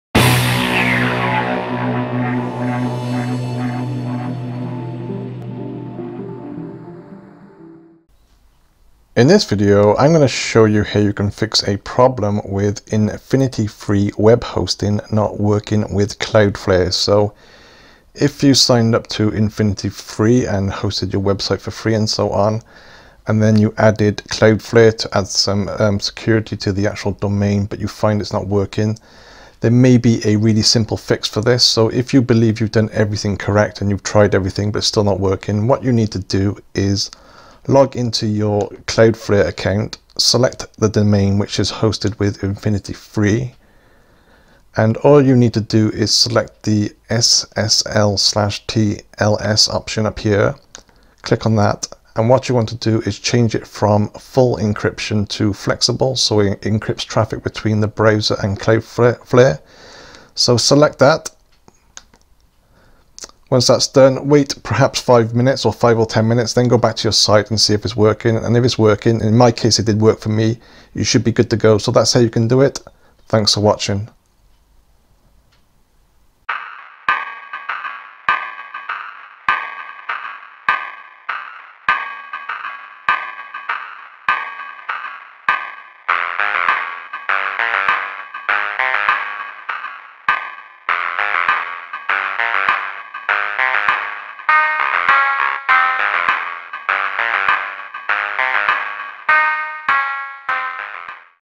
Cloudflare Not Working with InfinityFree Web Hosting Fix
In This Video I'll Show You How You Can Fix a Problem with InfinityFree Web Hosting not working correctly with Cloudflare.